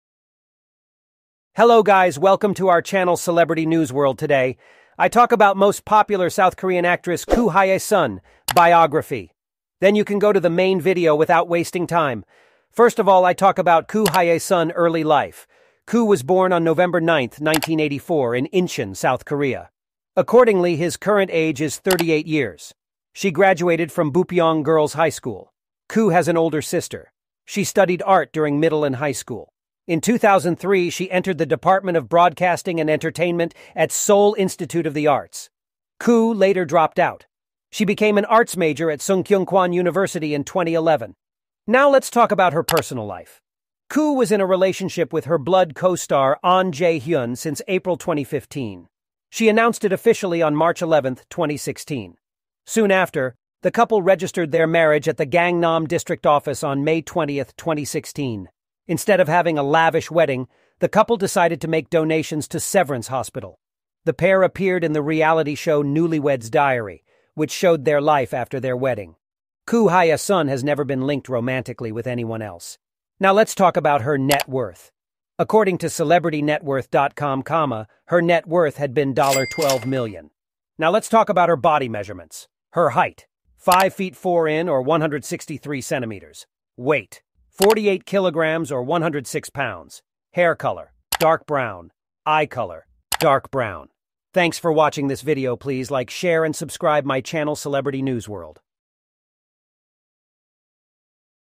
Ku Hye-sun biography in 2023 !
Image & video Credit : Google / Instagram..

Ku Hye-soon is a South Korean actress, director, and screenwriter who is known for her versatile acting skills and her work as a socially conscious filmmaker. She was born on August 9, 1981, in Incheon, South Korea.

Ku Hye-soon made her acting debut in 2001, and has since appeared in numerous films and television dramas, earning critical acclaim for her performances in works like "Sang-soo Im's "The Taste of Money" and "Kim Jee-woon's "A Bittersweet Life." She has also directed and written several award-winning short films, including "The Queer Movie Butterfly" and "Breath."

In addition to her work in the entertainment industry, Ku Hye-soon is known for her activism and advocacy on a variety of social issues, including LGBTQ rights and environmental protection. She is married to fellow actor Ahn Jae-hyun, and the couple has been active in supporting various causes related to children's welfare and mental health.